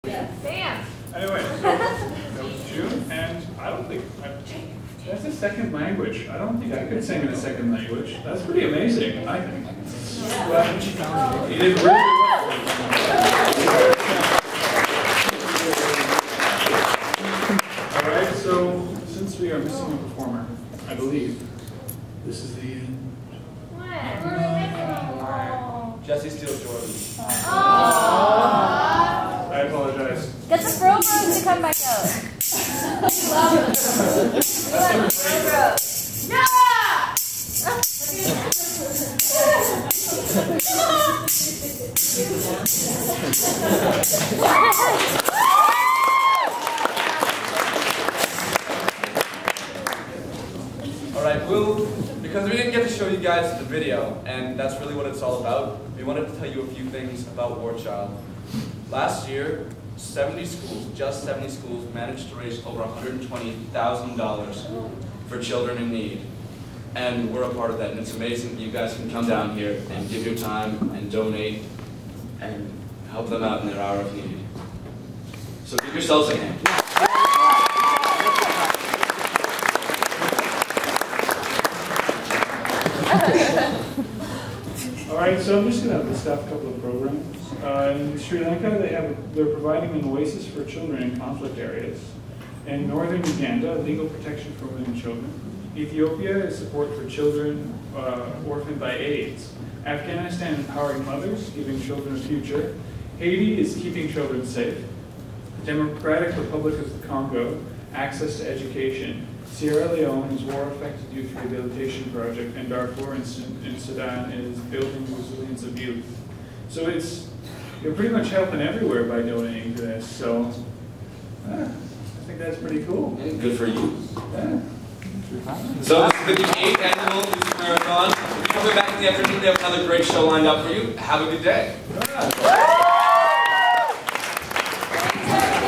W.L.S. Music Marathon '11 – 12 – The Hosts, pt. 2
Raw footage of the 2011 Music Marathon at W.L. Seaton Secondary School, Vernon, BC. Shot on June 2, 2011 by Graeme Higginson.